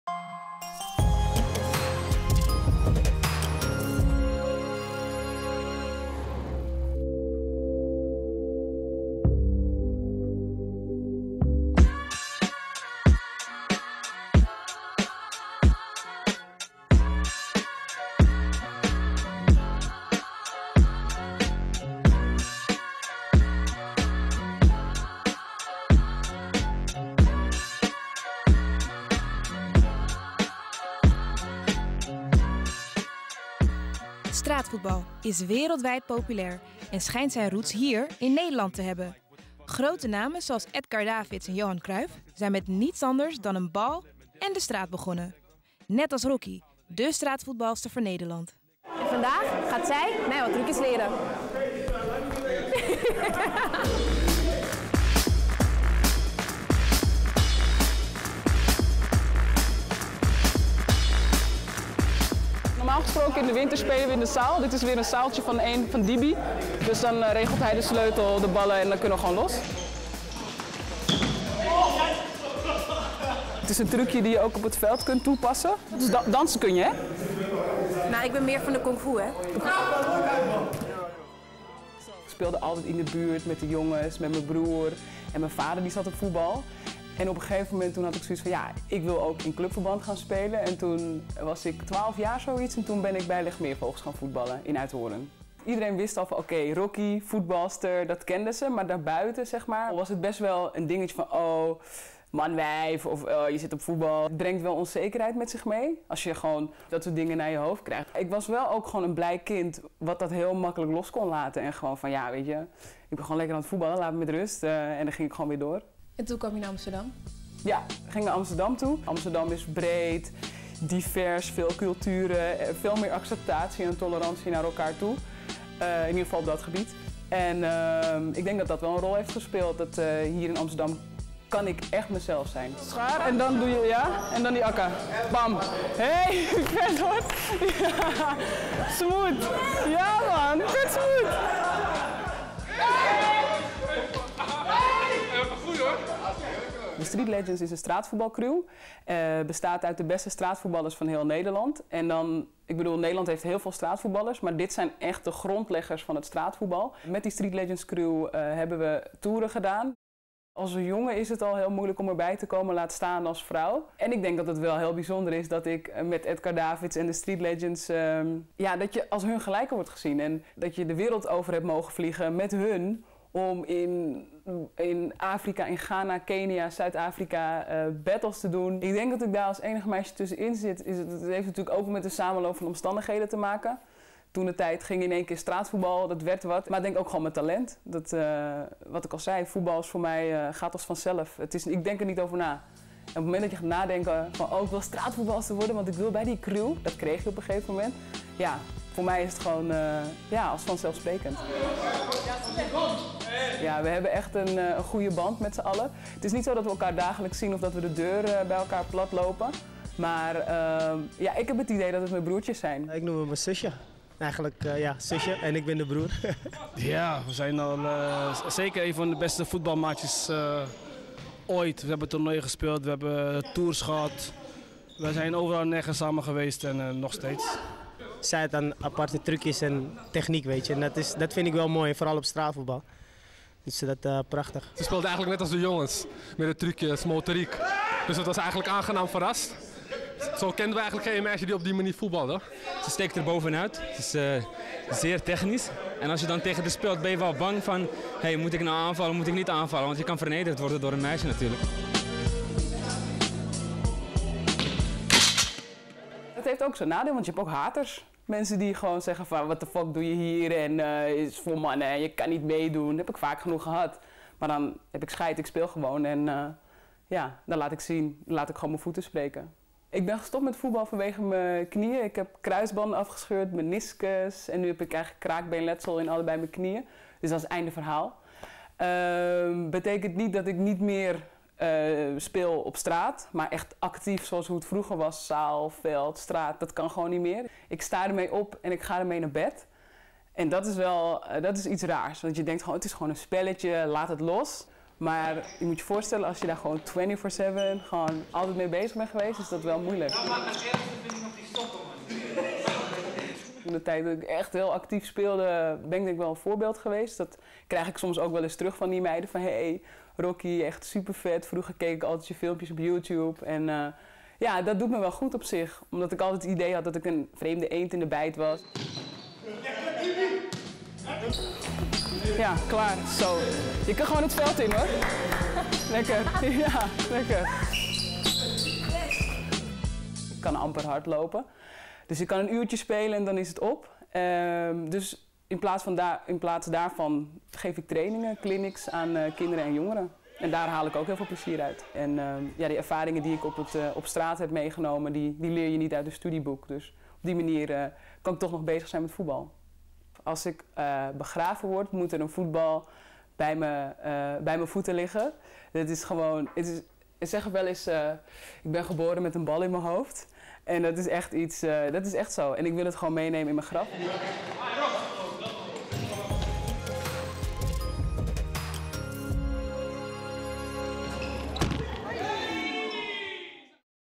130208 Straatvoetbal
Straatvoetbal is een sport die wereldwijd beoefend wordt. In Amsterdam vind je de grondleggers van deze activiteit. Voetbalster Rocky vertelt over haar ervaringen als enige vrouw in een straatvoetbalteam van mannen.